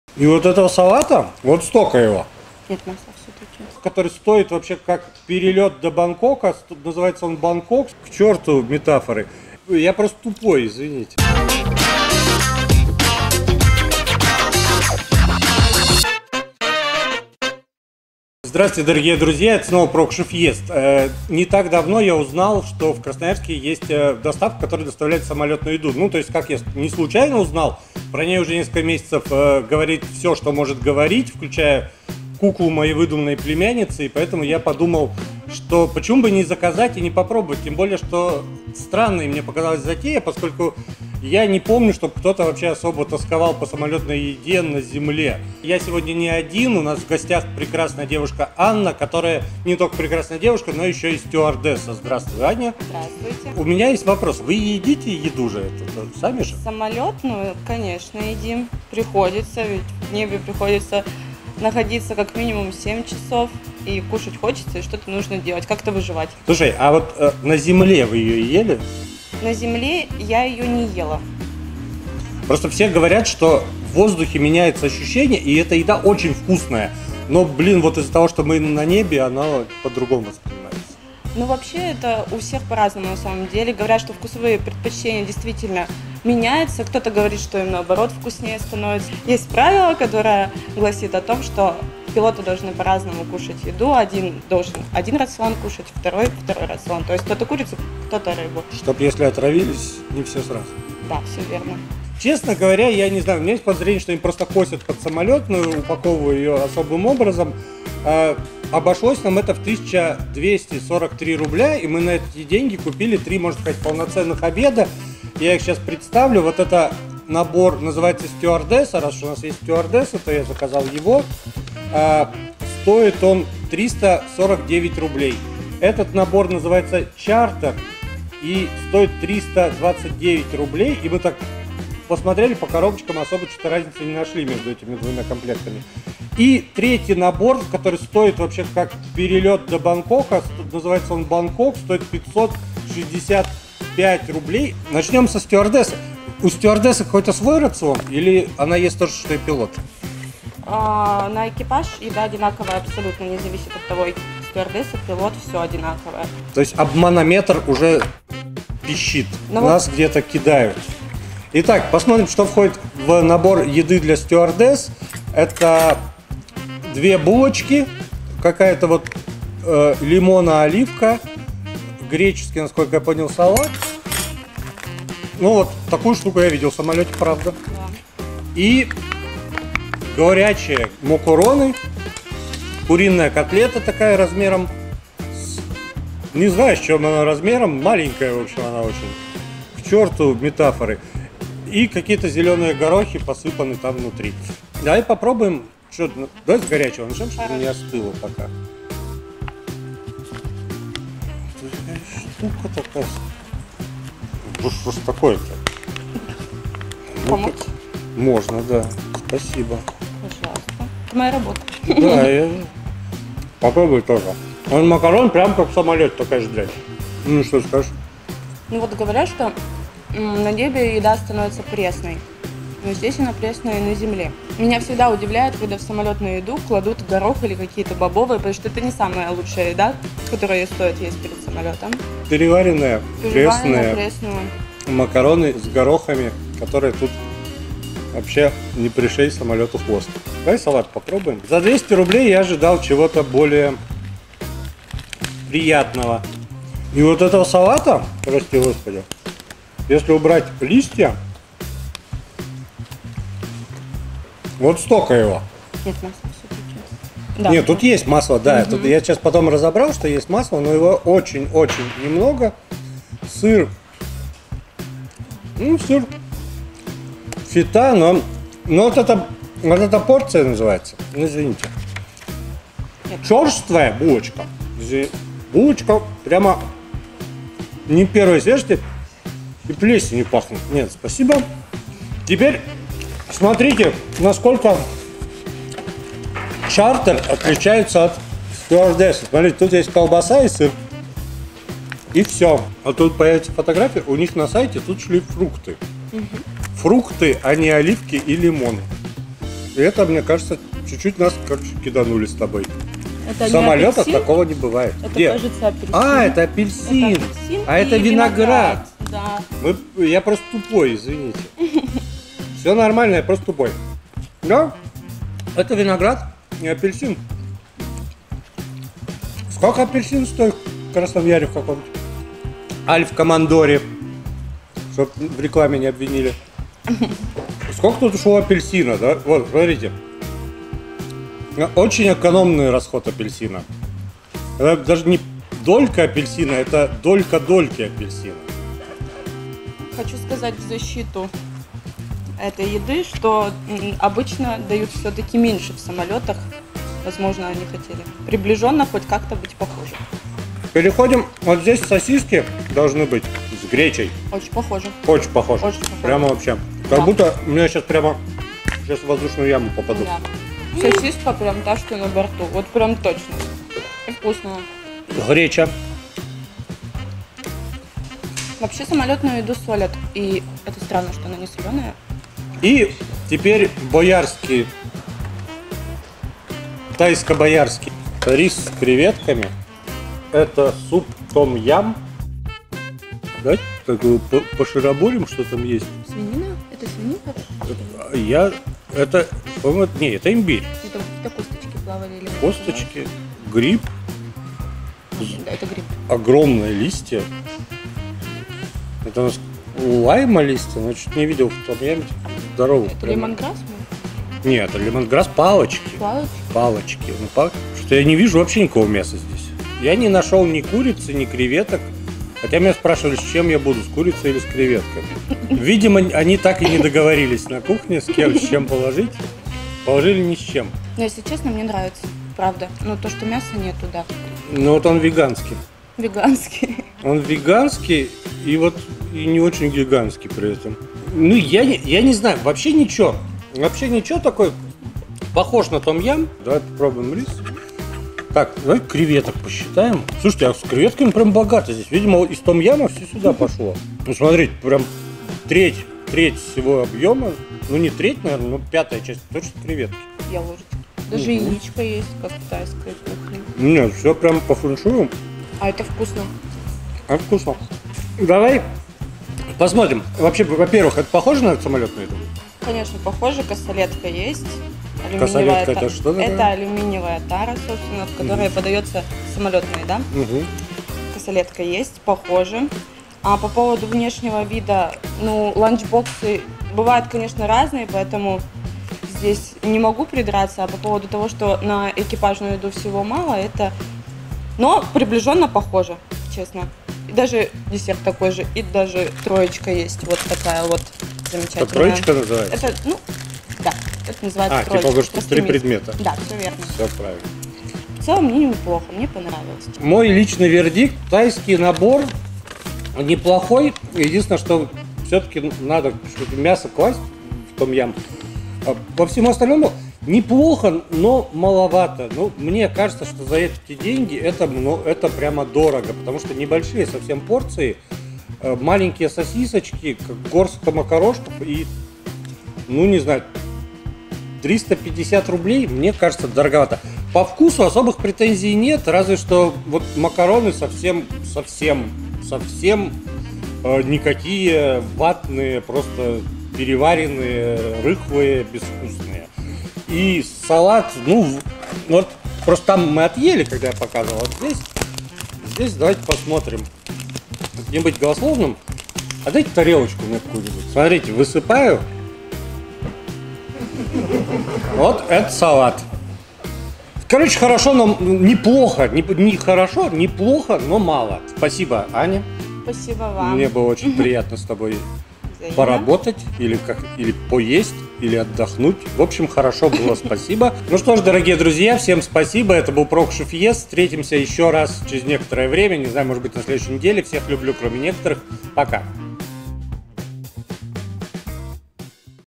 Прокушев и стюардесса едят (самолетную еду)
В Красноярске появилась доставка самолетной еды, и Василий Прокушев просто не смог пройти мимо.

А чтобы обзор получился совсем честным, мы пригласили на дегустацию настоящую стюардессу! ✈